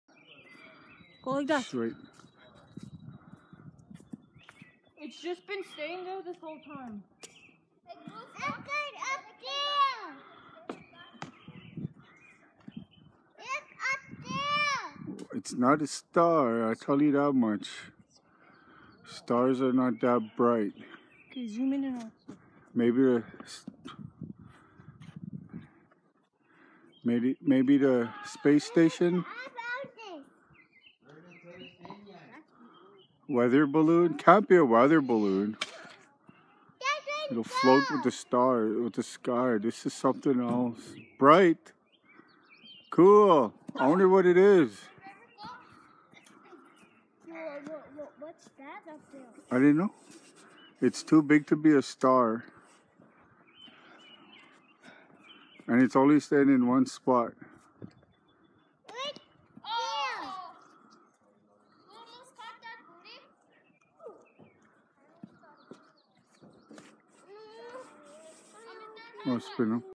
UFO over my house
The UFO was hovering over my house as me & my kids where playing baseball it appeared to look like a metal sphere made no sound and their was no stars out